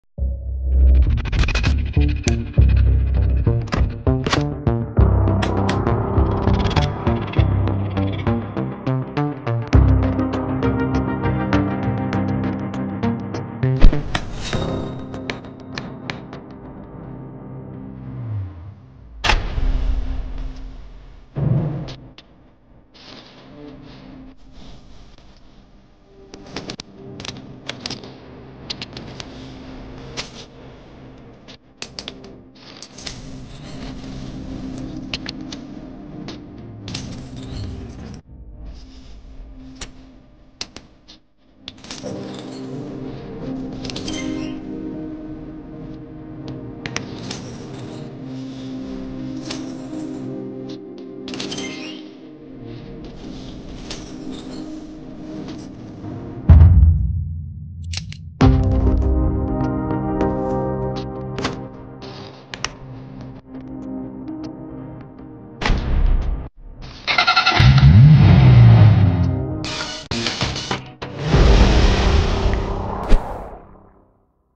Redmi K50 Gaming Edition Teardown and Making Sports Car
Redmi K50 Gaming Edition Teardown Video Out Now